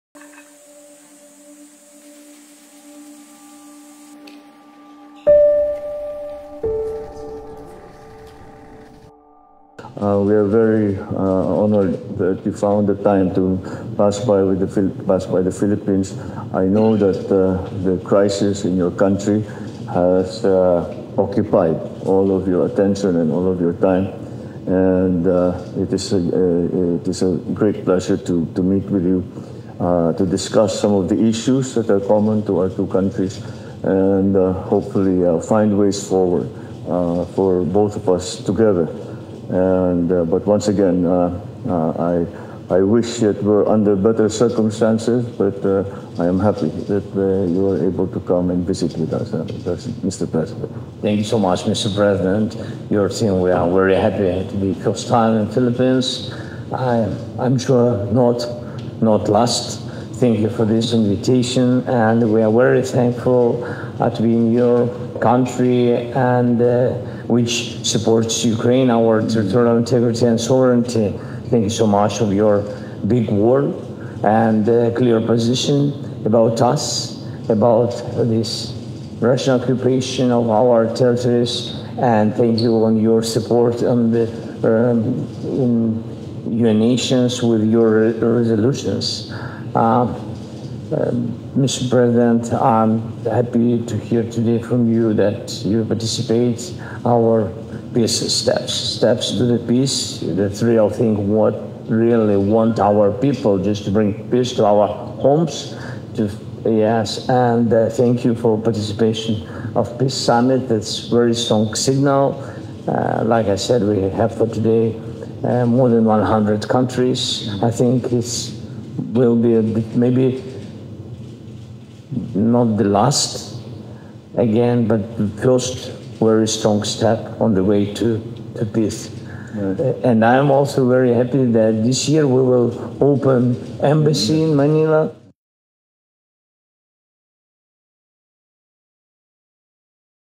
Президент України відвідав Філіппіни
Прибув до Філіппін та вперше в історії наших двосторонніх відносин зустрівся з Президентом Фердинандом Маркосом-молодшим.

Вдячний Філіппінам за підтримку суверенітету й територіальної цілісності України, за чітку позицію щодо агресії Росії проти нашої країни, а також за підтримку важливих резолюцій ООН. Україна має намір відкрити посольство в Манілі цього року для зміцнення двосторонньої співпраці.

Обговорили інавгураційний Глобальний саміт миру та важливість представлення на ньому країн Південно-Східної Азії. Цінуємо, що Філіппіни візьмуть участь у Саміті.

Також обговорили двосторонню співпрацю, зокрема експорт української аграрної продукції до Філіппін.